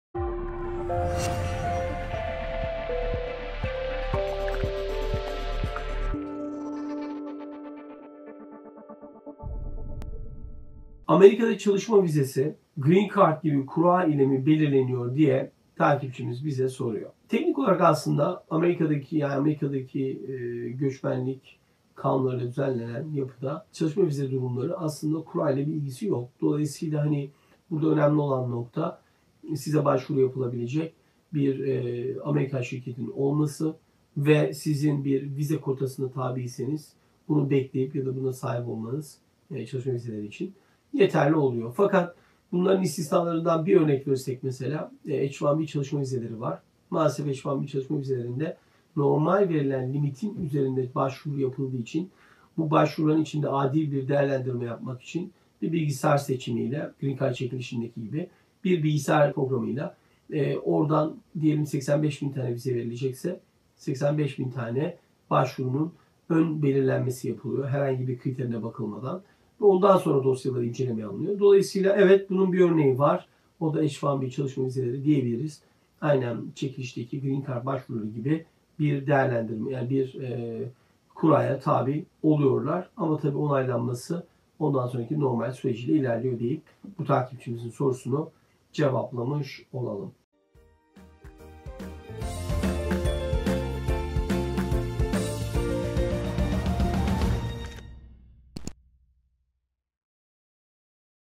AMERİKA ÇALIŞMA VİZELERİ KURA İLE Mİ BELİRLENMEKTEDİR ?
Soru: Amerika çalışma vizesi, Green Card gibi kura ile mi belirleniyor ?